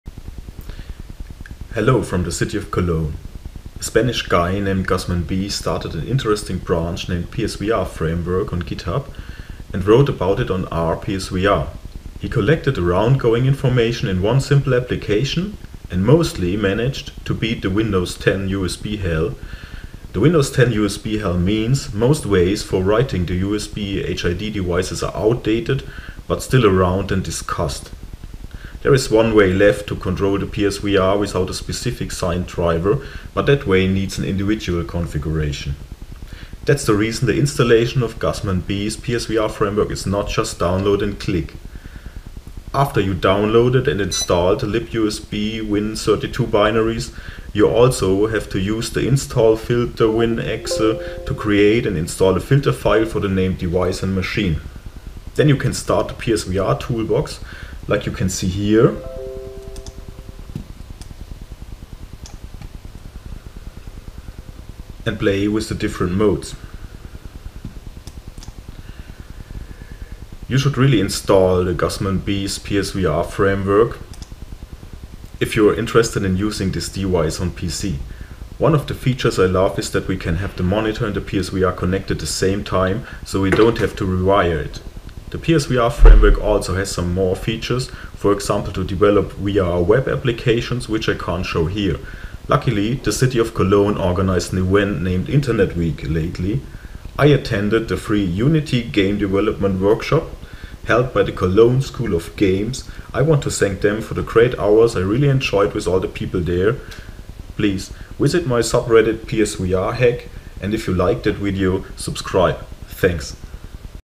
r/PSVRHack on Reddit: Connect the Playstation VR to a Windows PC with the gusmanb PSVRFramework
Use the gusmanb PSVR Framework to switch the Playstation VR modes and connect a second monitor in parallel.